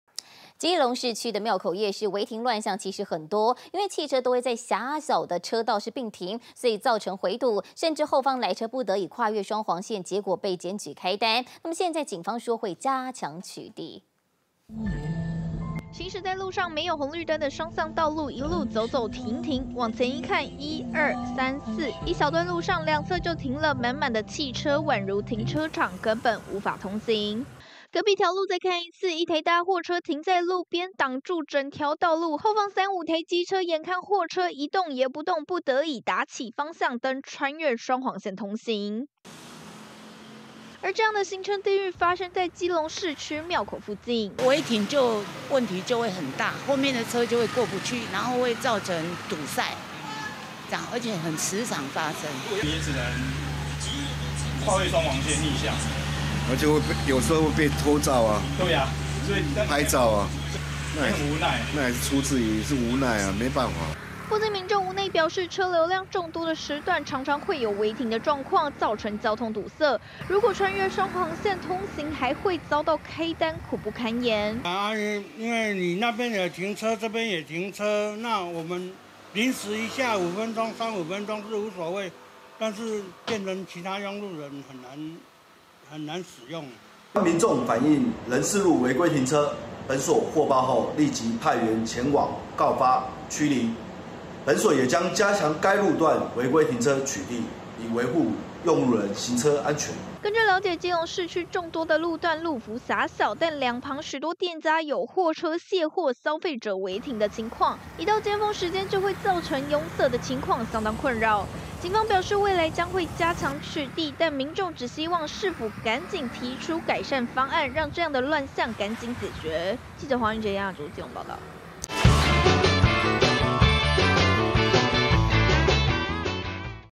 違停亂象多！ 基隆廟口道路雙向「滿滿違停」 宛如停車場
基隆市區廟口夜市違停亂象層出不窮！汽車在狹小車道併排停車，造成堵塞狀況嚴重，宛如停車場，後方車逼不得已跨越雙黃線才得以通行，還可能會被開單。這些違停亂象讓附近民眾苦不堪言！由於投訴太多，警方也出面回應未來將會加強取締。